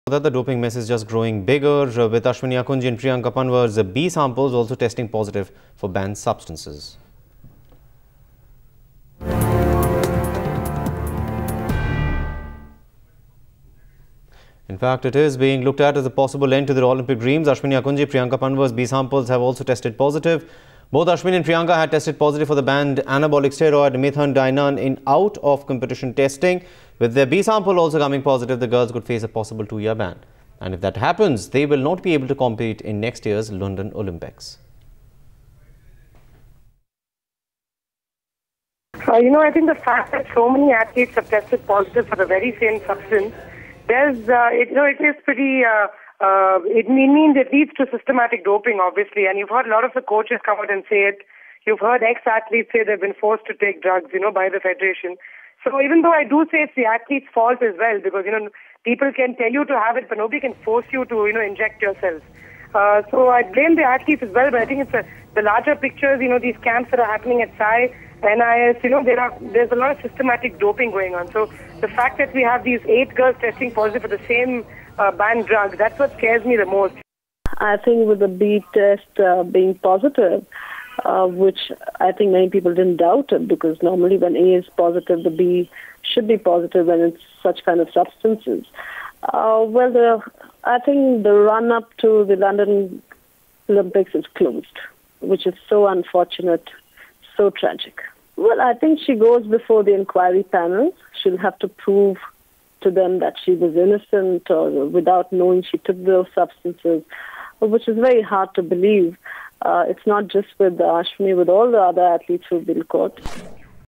Ashwini, Priyanka's 'B' samples also test positive
Dope tainted athlete Ashwini Akkunji's London Olympics dreams are virtually over as her 'B' sample also tested positive for anabolic steroids and she now faces the prospect of a two-year ban for her offence. Another quartermiler Priyanka Panwar's 'B' sample also tested positive for anabolic steroids. Download the Times Now India's Election HQ app and get all the election info at one go. Social Media Links